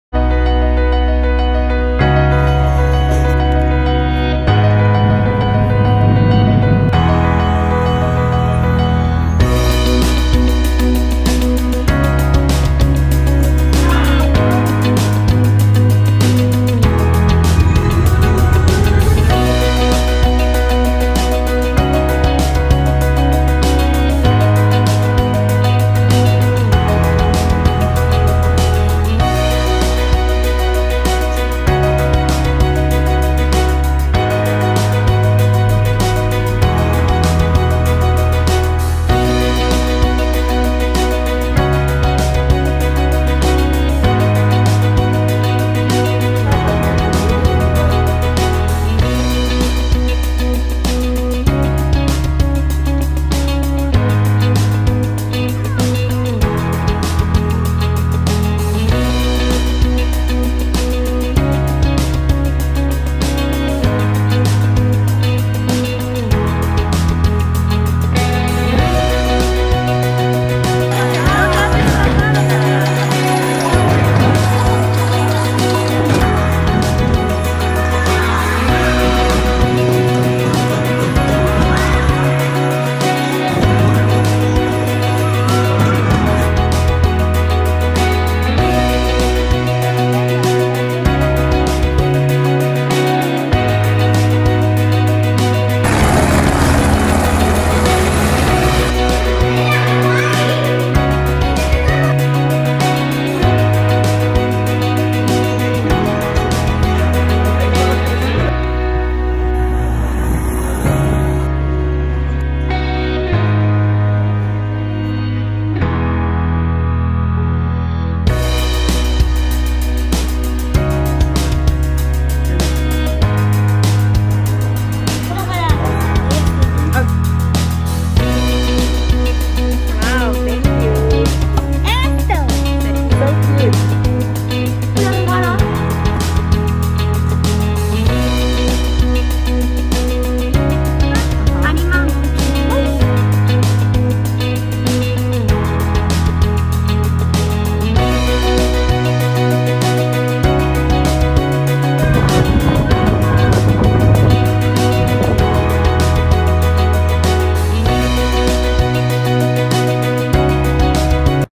A year in the life of a NALCAPer
Here's a glimpse of what a year in your life as a NALCAPer can look like.